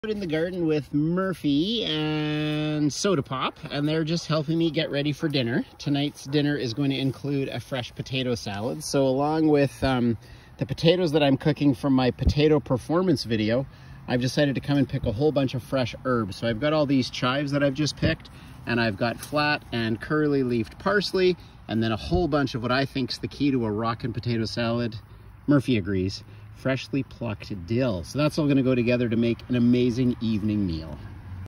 Fresh Herbs - Potato Salad
To bring a potato salad to life - the best injection is FRESH HERBS!